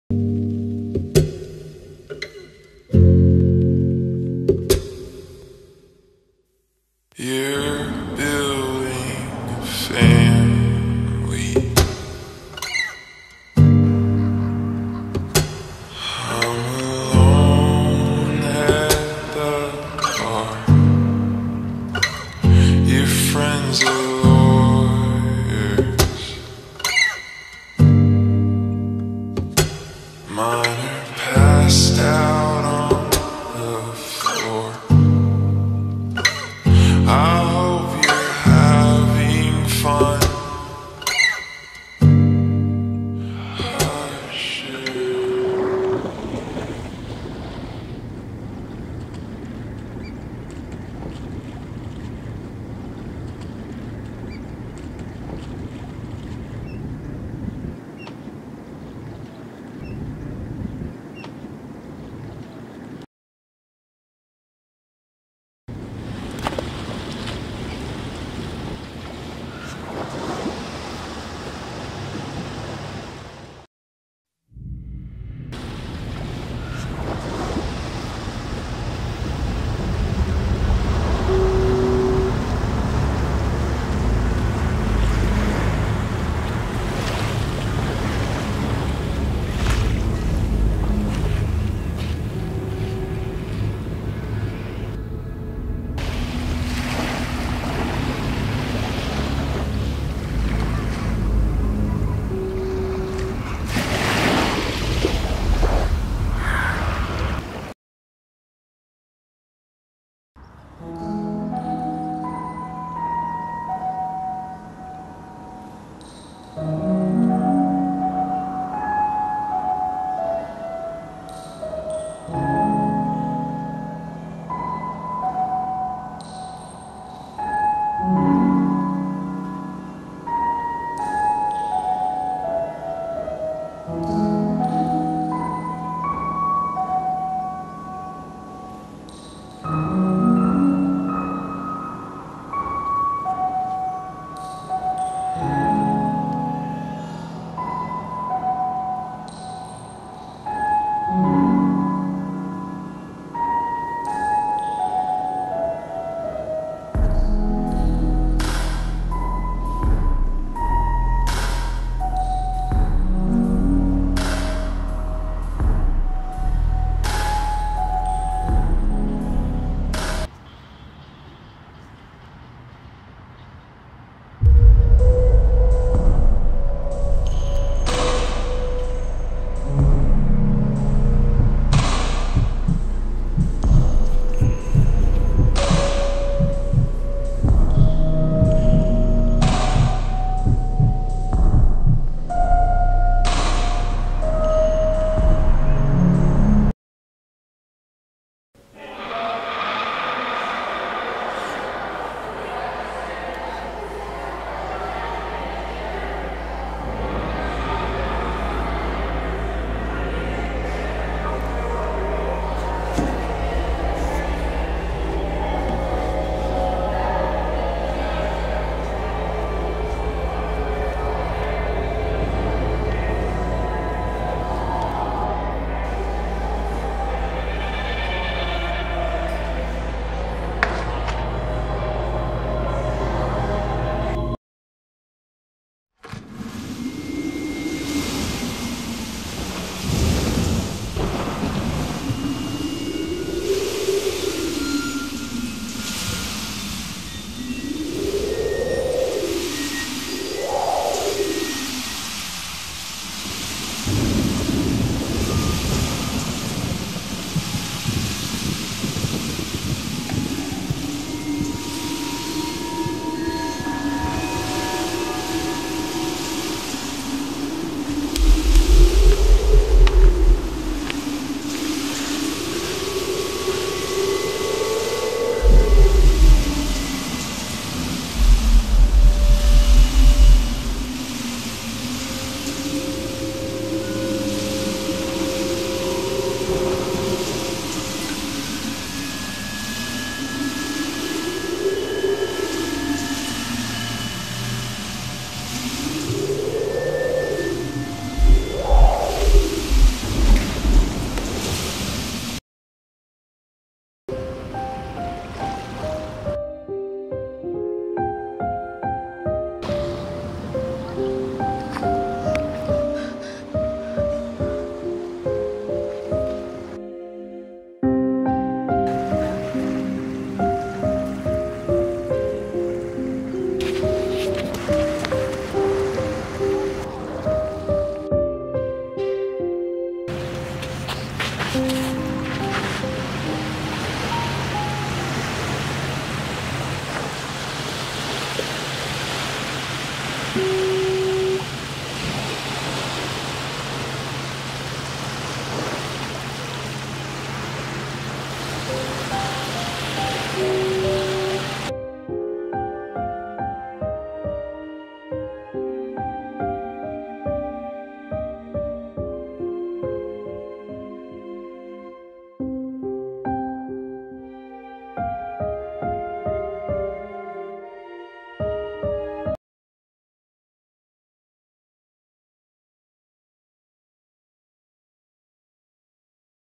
ROSE OF MY DREAMS ; a short film
WHEN AMELIA'S BEST FRIEND DIES SHE BEGINS TO BLAME HERSELF FOR HER FRIENDS DEATH. AFTER VISITING HER FRIENDS GRAVE AND LEAVING ROSES SHE FINDS ONE LAYING ON THE SHORE. WHEN PICKING IT UP SHE GETS SUCKED INTO A DREAM WHICH SOON ENDS UP TWISTING ITSELF INTO A NIGHTMARE.
Follow the cast! (Instagram)
actor-Isabel Ramirez - @isabelramirez._
actor-Elizabeth Wong - @elizabeth.wongg

Directed by Saniyah Syed - @imsaniyah